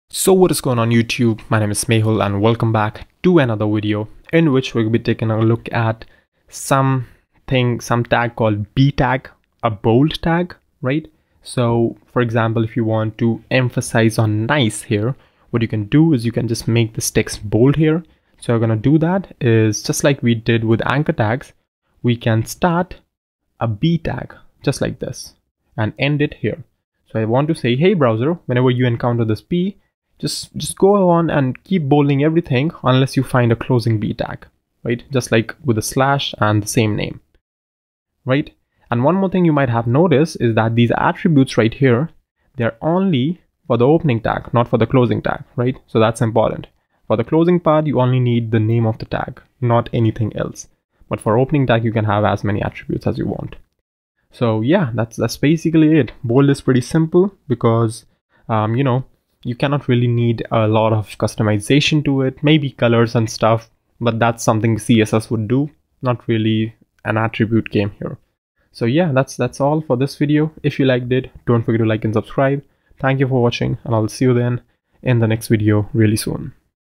#10: Bold (b) tag - HTML5 + CSS3 Frontend Web Development Tutorials
In this video let's discuss how bold (b) tag works in HTML